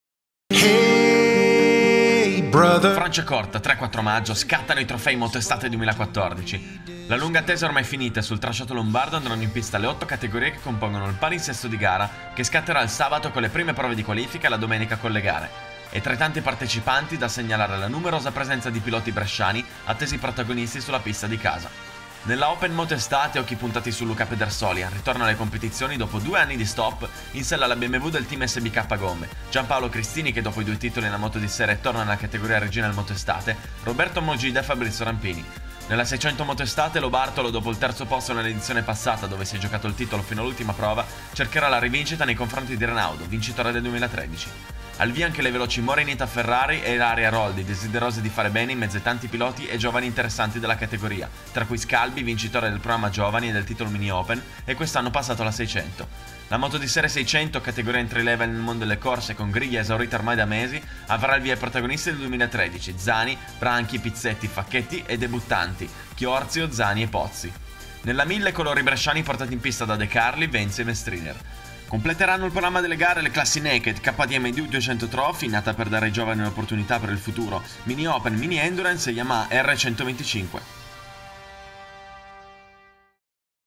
Presentazione Motoestate Franciacorta 3-4 maggio
Prendono il via i trofei Motoestate dal circuito di Franciacorta! 8 categorie al via per una domenica spettacolare e ricca di adrenalina ed eventi nel Paddock!